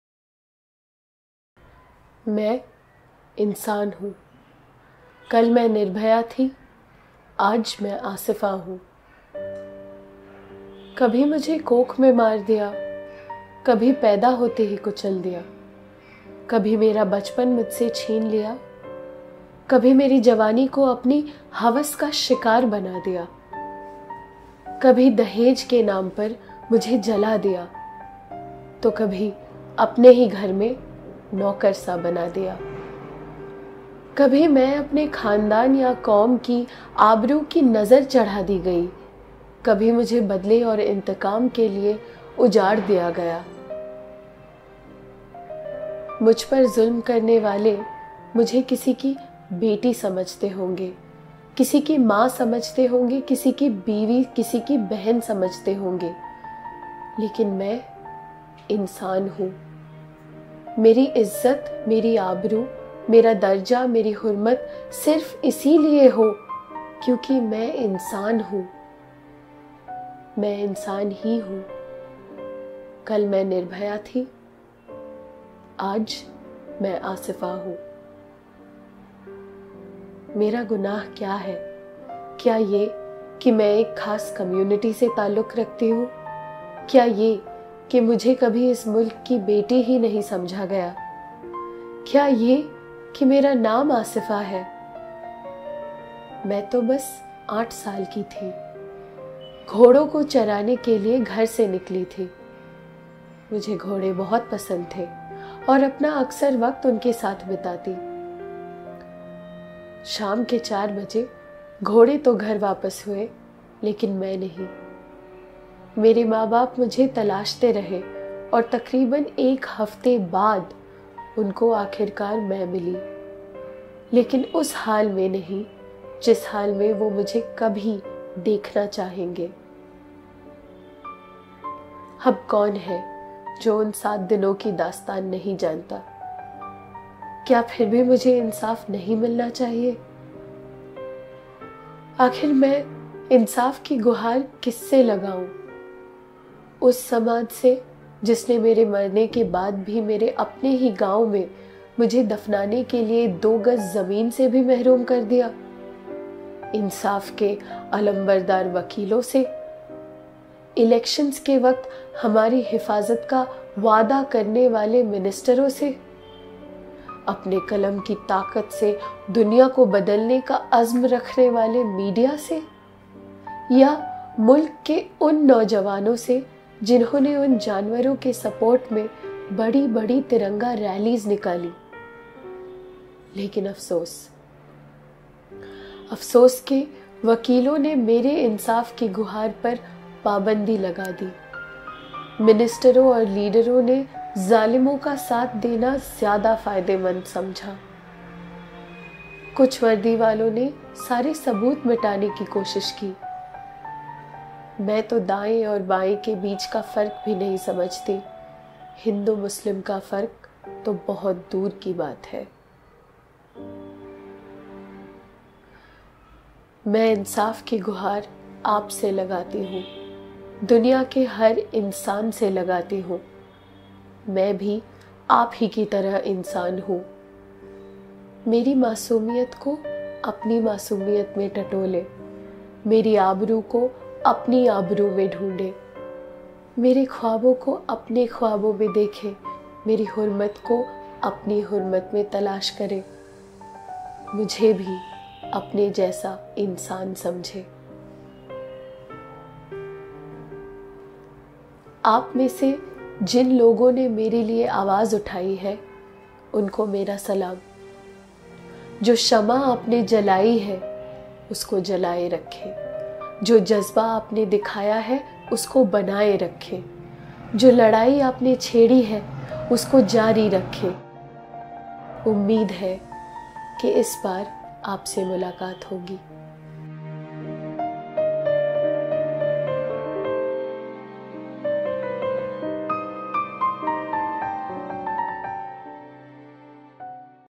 Asifa ke Mann ki baat
This short video(4.5m) by Tajalli Productions takes account of various atrocities that occur on Indian women and urges people of all faiths to work towards ensuring justice to them.